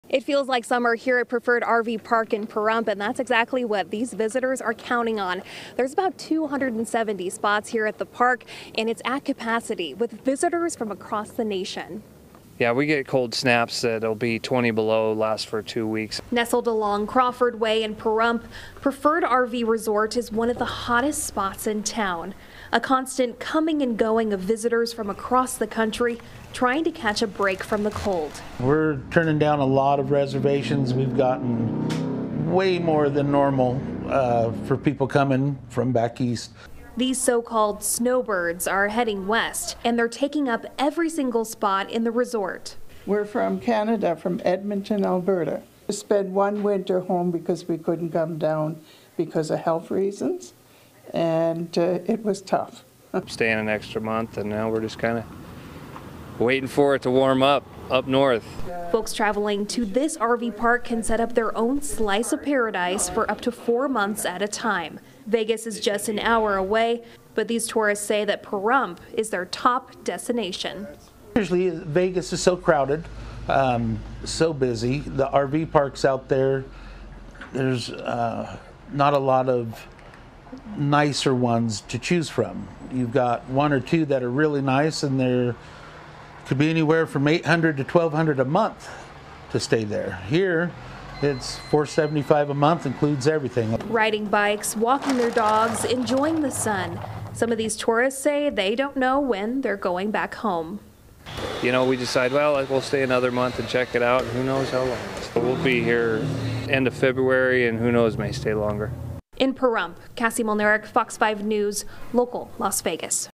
Snowbirds flock to Pahrump
The polar vortex is driving business in Pahrump.